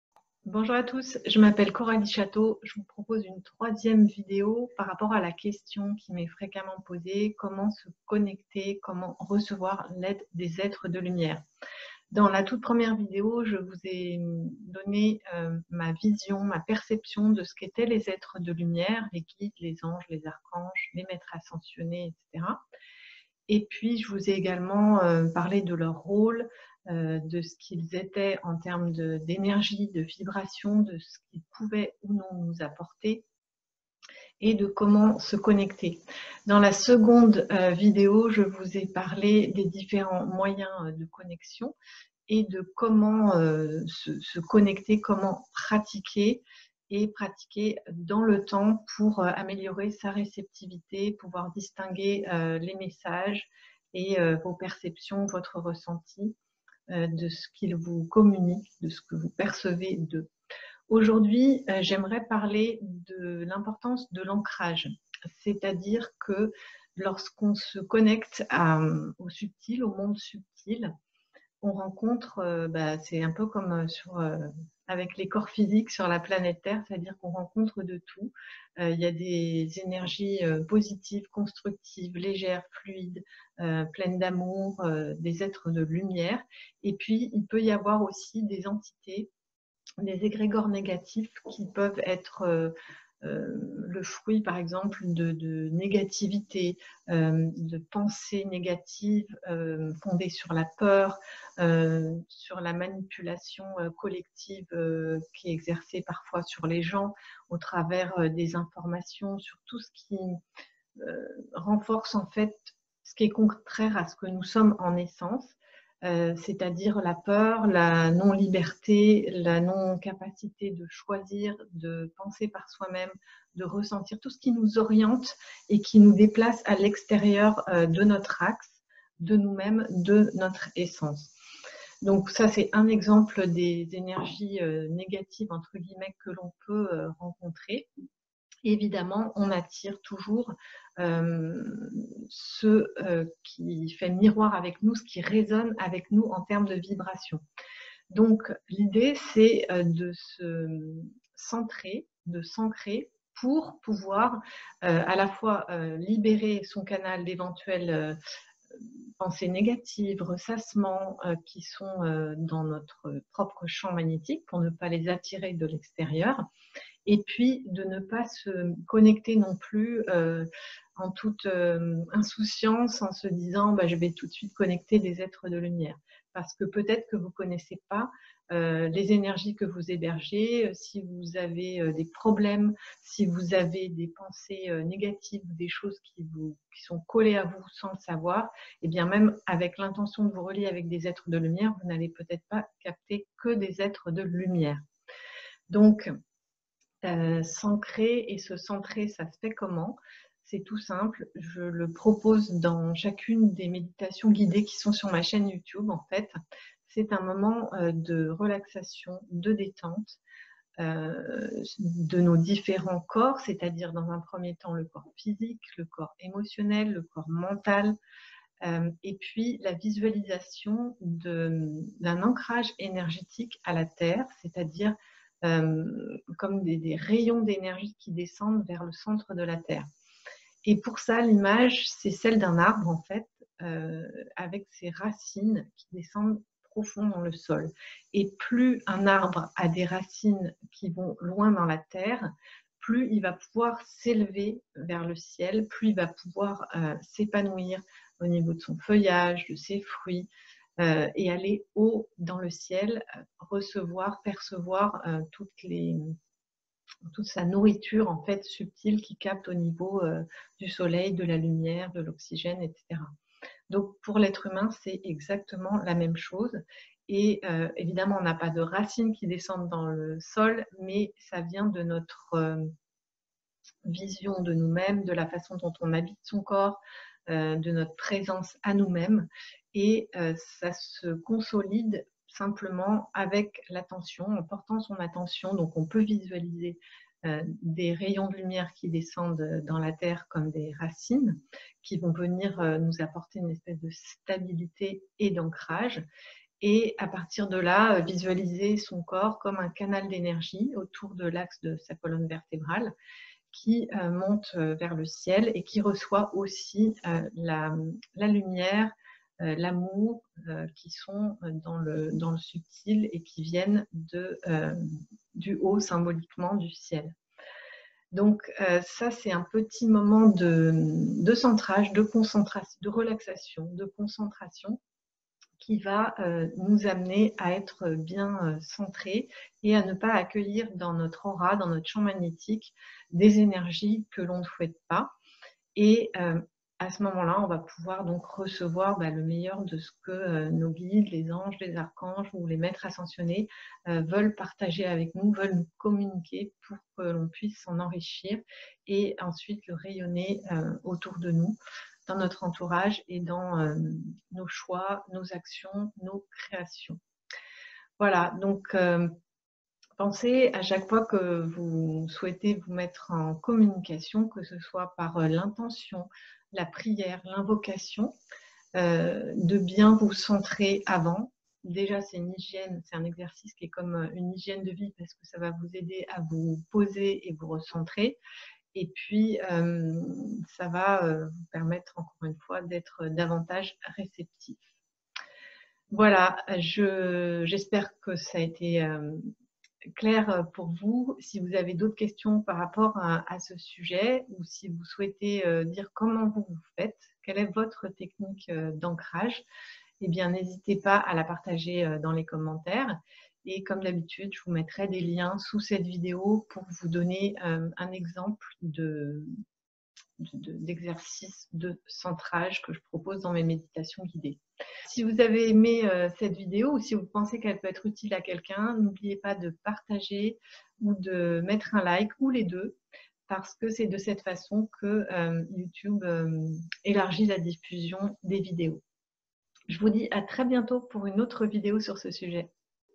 Comment recevoir l'aide des Êtres de Lumière Vidéo 3
Se centrer et s'ancrer pour mieux recevoir l'aide des Guides de Lumière.